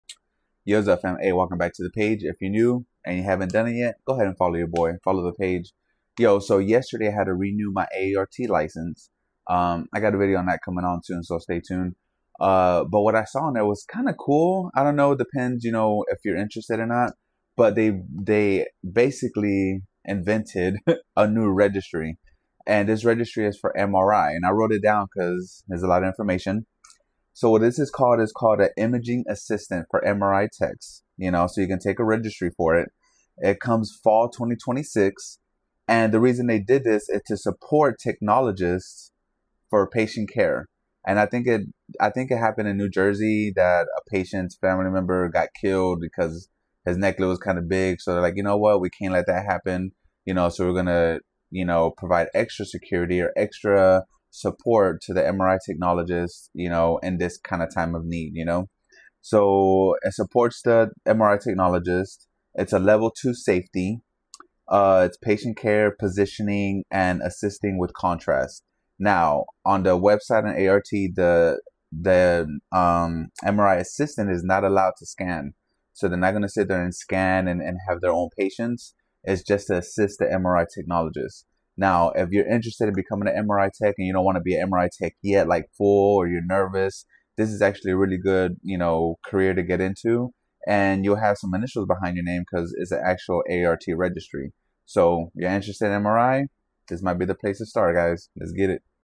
NEW MRI 🧲 REGISTRY COMING 2026‼️👀
WELCOME 🙏🏽 FAM

ON THIS SHORT I’M TELLING YOU THE EXCITING NEWS ABOUT A NEW MRI 🧲 REGISTRY THAT ARRT WILL BE PROVIDING STARTING FALL OF 2026‼️